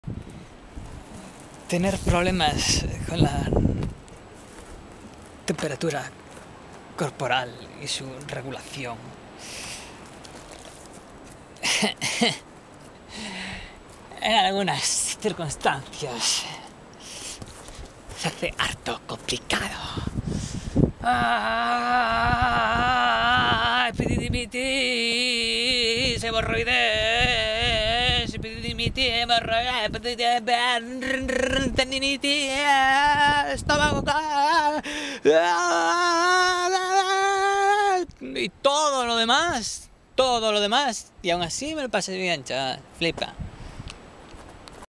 Contra viento y marea
La regulación de la temperatura corporal puede verse descompensada por estar en sitios con mucho sol, sin sombra y con mucho viento (y si te has tomao 2c-b  más). La epididimitis duele, las hemorroides también, el agua escasea, problemas de estómago, tendinitis en los dos tendones de aquiles, sin comer desde hace casi 24 horas, trepando como un macaco... Y que me quiten lo bailao.